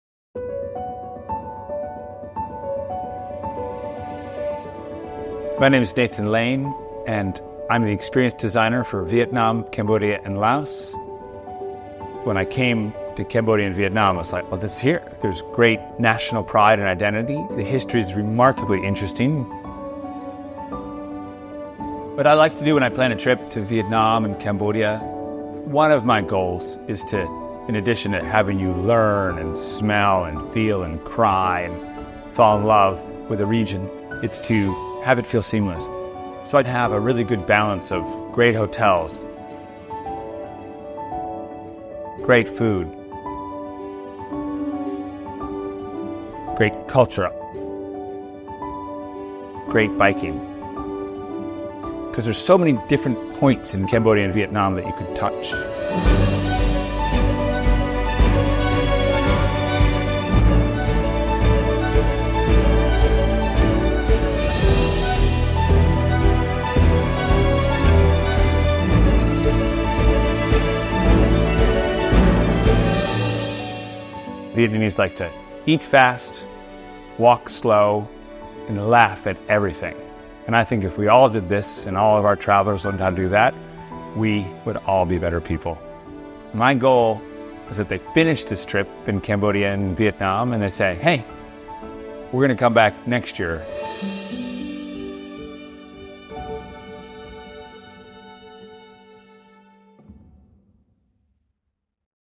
Cambodia & Vietnam Biking
Exploring Vietnam and Cambodia never gets old. Every new visit entices us the same way it did on our first bike tour there decades ago.

This pocket of Southeast Asia transforms the perspective of Nathan Lane, our Experience Designer for the region, on every trip too.

To really get to know the culture, history, food and people here, the best way to do it is our favourite way: by bike.